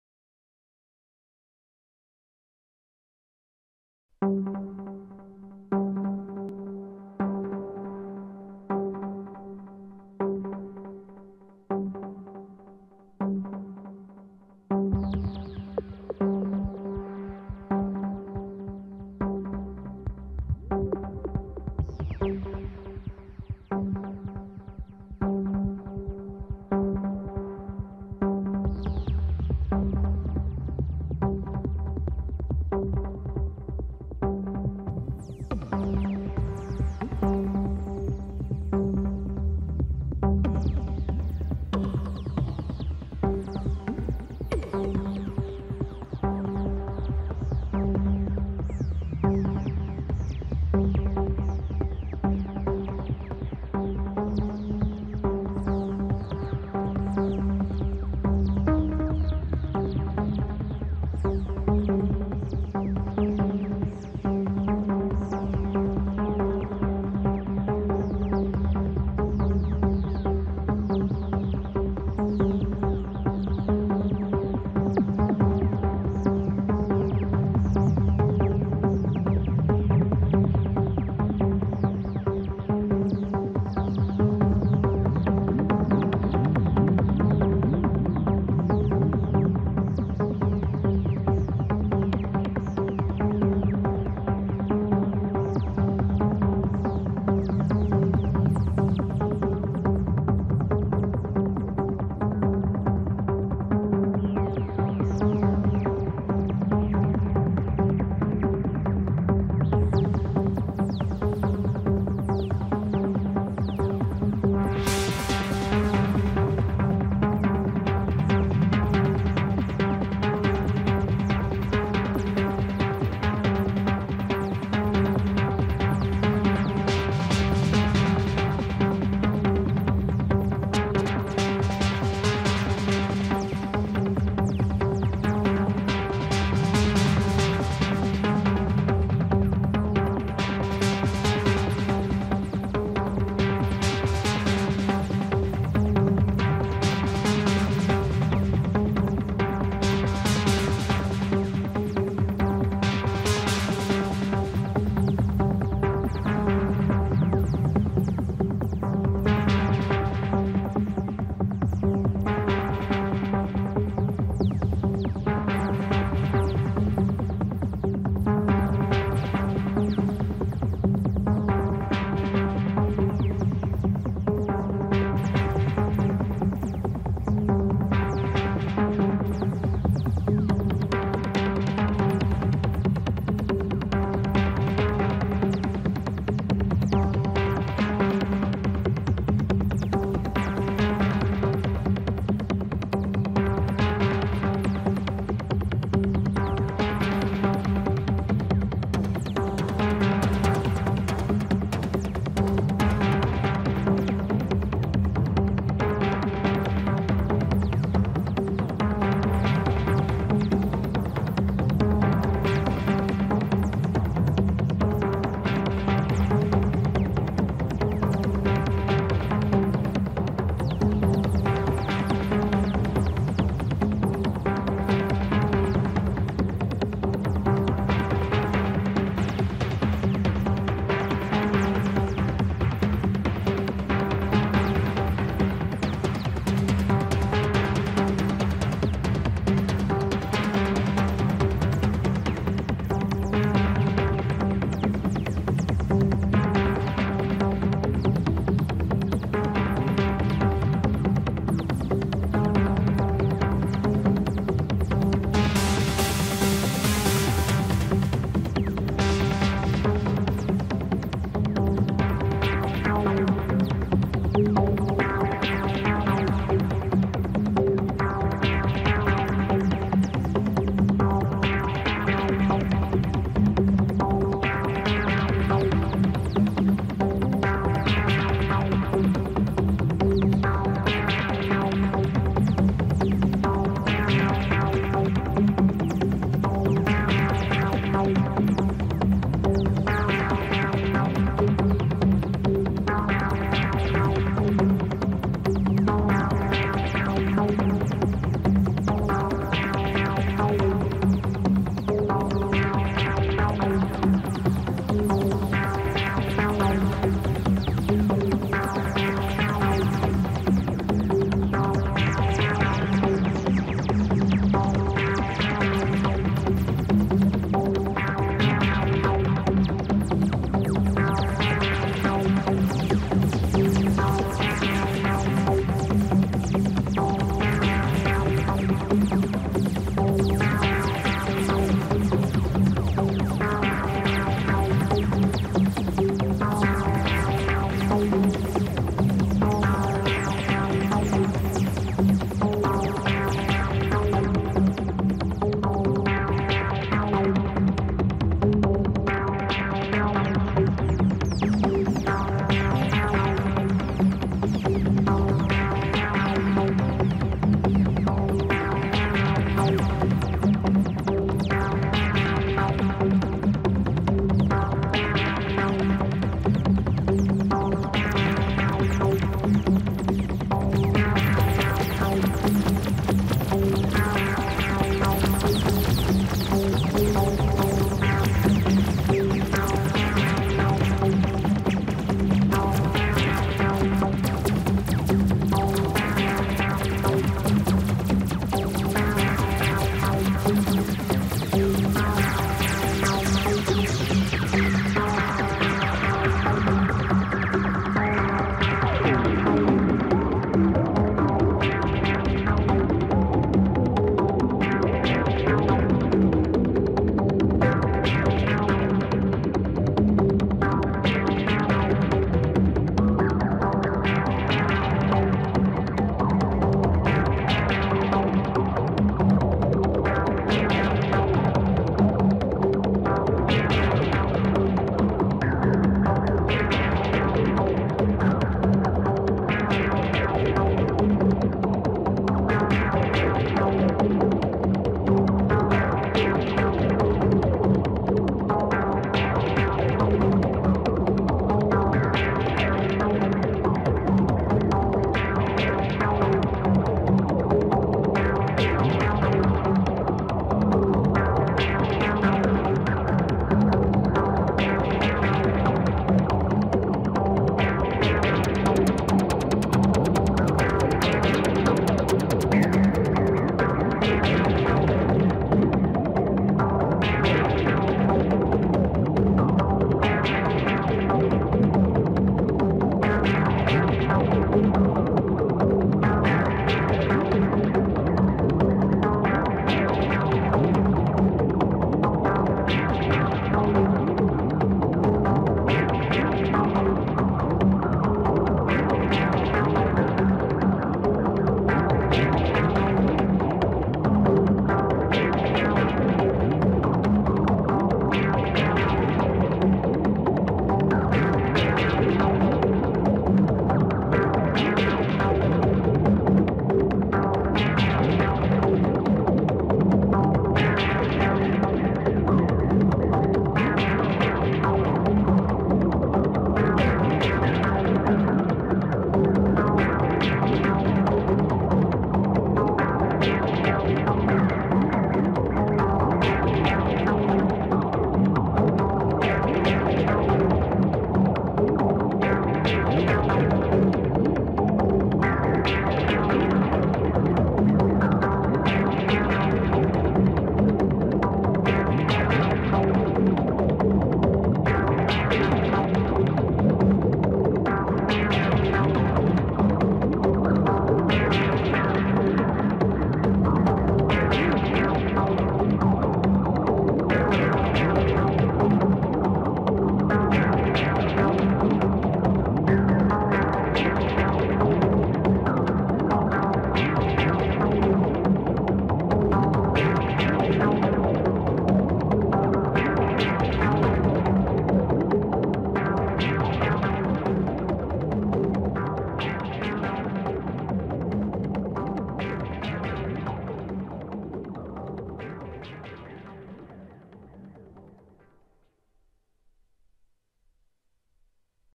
phrozenlight - how to make a sequence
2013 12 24

here is a short video where I demonstrate how I make a sequence

Doepfer MAQ 16/3 & Novation KS-rack

1 row 6 knobs active sends to channel 1 / patch 1
2 row 16 knobs active sends to channel 2 / patch 2 & 3
3 row 4 knobs active sends to channel 3 / patch 4